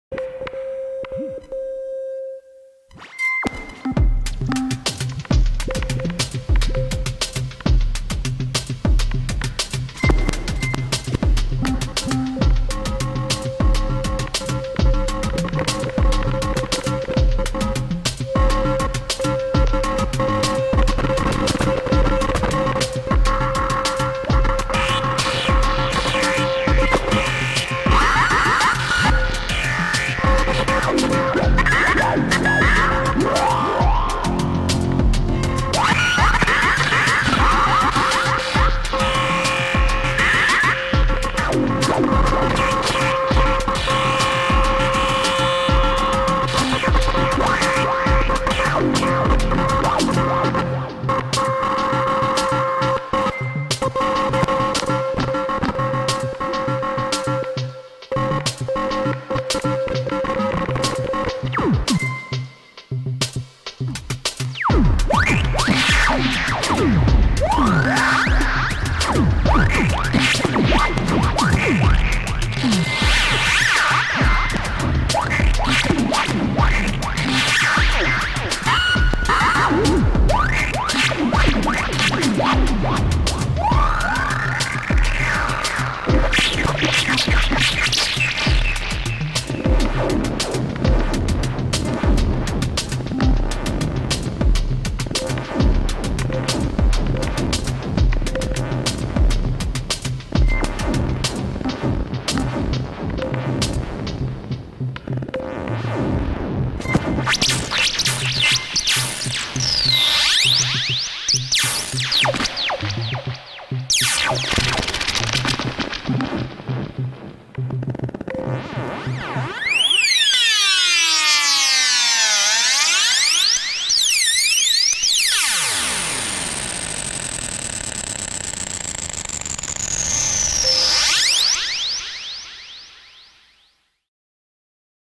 Nonlinearcircuits Ian Fritz Hypster
Just finding getting Hypster together. Had a what is this thing doing moment, but after getting it patched, this is an awesome modulation source.
Here the 8 outputs are modulating a bunch of things. Log FM and PWM on the CEM3340 VCO, filter and band pass on the BBX291 Filter, triggering Sly Grogan to trigger Qu-Bit Surface, and sending modulation there and to Feague which is doing its thing to Plaits.